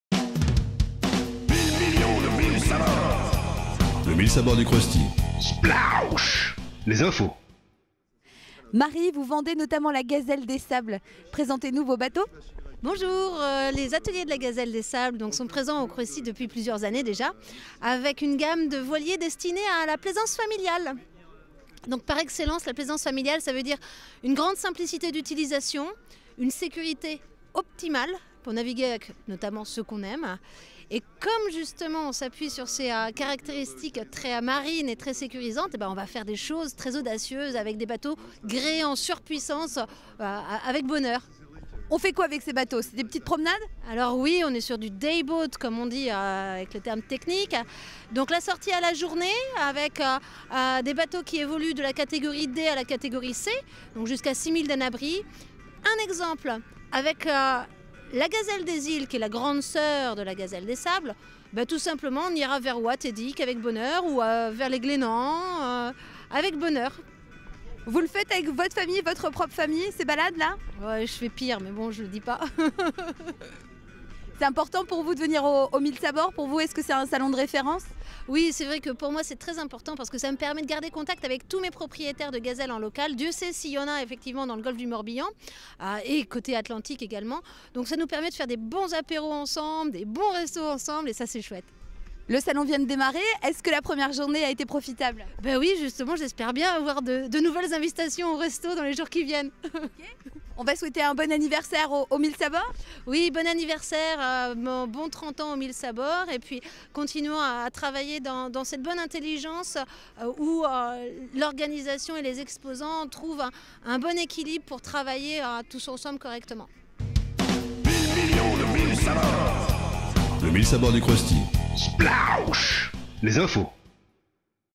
Marie Besnié des Ateliers de la Gazelle des Sables
Le Mille Sabords (Morbihan), plus grand salon du bateau d’occasion d’Europe.

30 Mille Sabords... et 30 % des bateaux vendus !

Le Mille Sabords clôt sa 30e édition anniversaire sur un résultat plus que satisfaisant : en quatre jours, particuliers et professionnels ont mis en vente plus de 800 unités, dont 200 à 250 ont trouvé preneurs. Les best-sellers : les unités à moteur, à moins de 30 000 €. Cerise sur le gâteau (d’anniversaire...) : des pontons animés et baignés de soleil en journée, exceptionnellement illuminés en soirée... Et des acheteurs venus par milliers, confirmant le statut définitivement à part du plus grand salon du bateau d’occasion d’Europe.

Réalisation (captation & montage) Jean-Thierry Debord
Interview Eric Ligué & Nadia Lorjoux